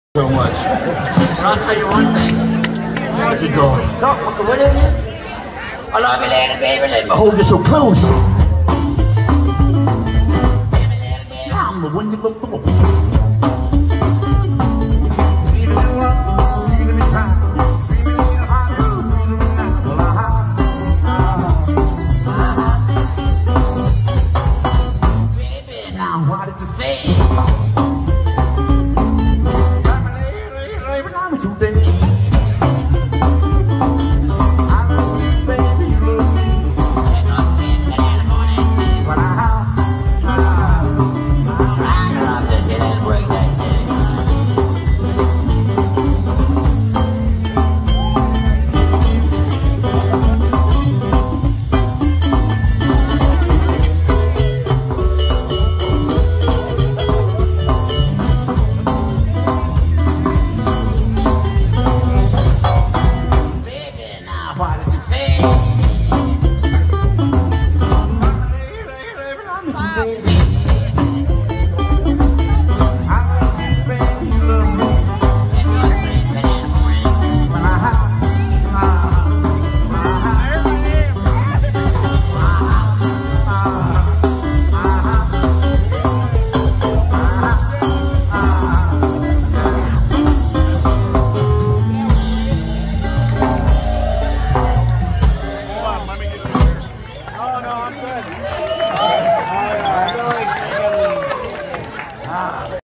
Bloodshot Bill & Friends | Symco Hotrod & Kustom Weekender 2022 | Symco, Wisconsin
Bloodshot Bill & Friends

Bill Williams - Vocals, Guitar
Bobby Trimble - Drums
Todd Wulfmeyer - Upright Bass

Symco Hotrod & Kustom Weekender 2022

Symco, Wisconsin